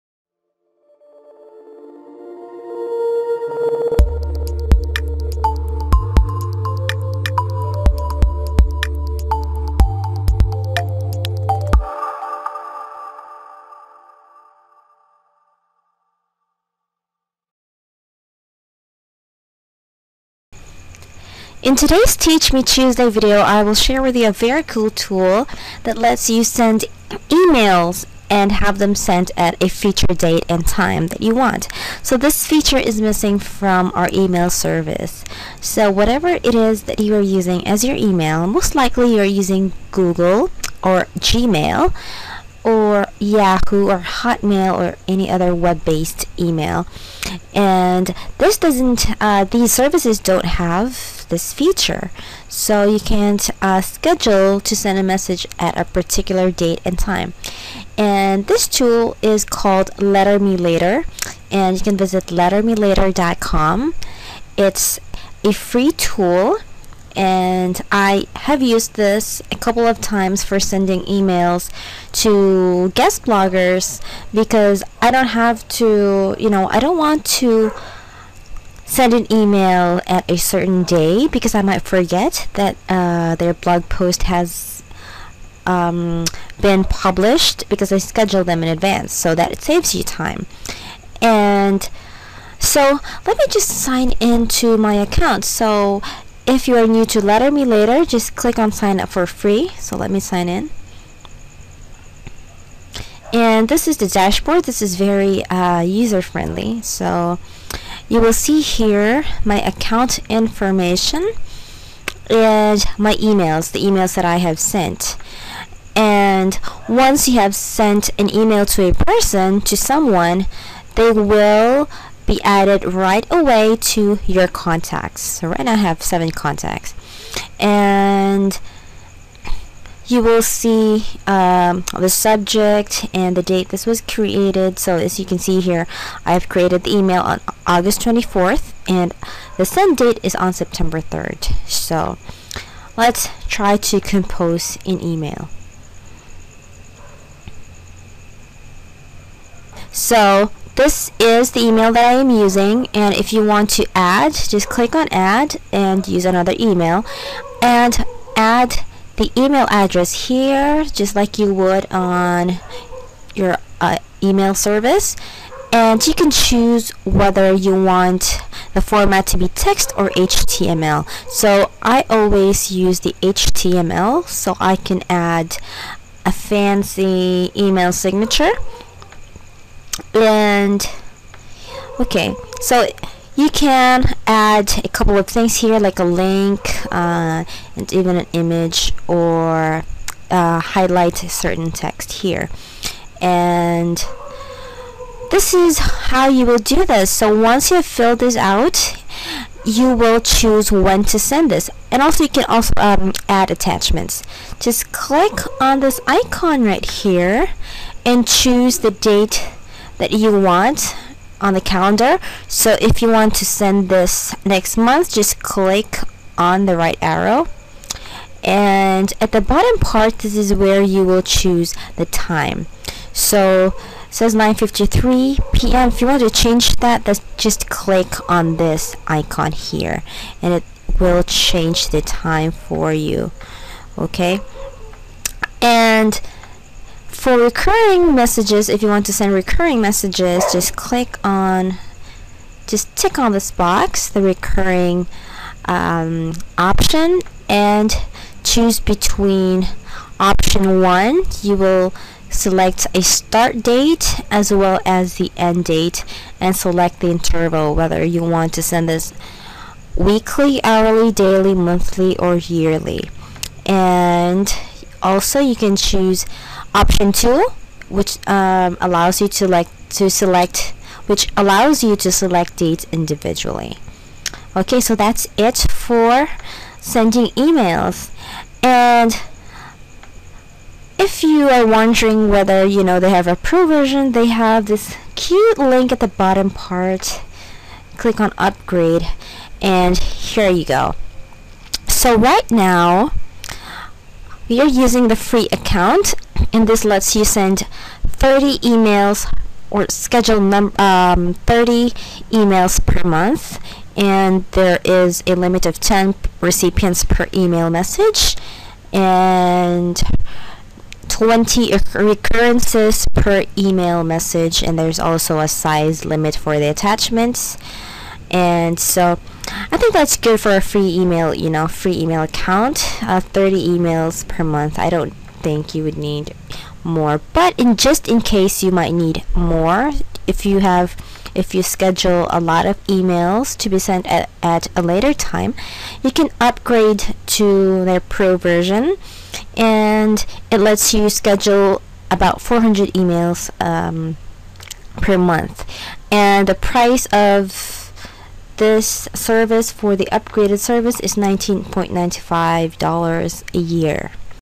How to Send Personal and Business Emails at a Later Date
The one missing feature in most email services is the ability to send emails at a later date and time. In this video, I will share with you a free tool that allows you to schedule emails and have them send on a date and time that you choose.